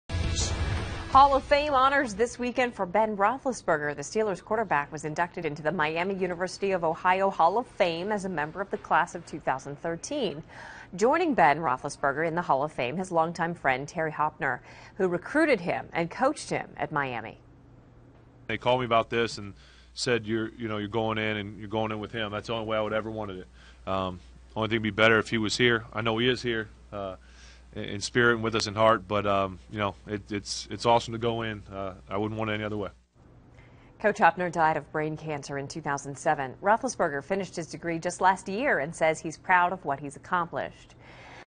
Roethlisberger goes into Miami University Hall of Fame with late coach
Pittsburgh Steelers quarterback Ben Roethlisberger was inducted into the Miami University Athletics Hall of Fame. His college football coach, Terry Hoeppner, was inducted posthumously with Roethlisberger in the Class of 2012-13. Roethlisberger finis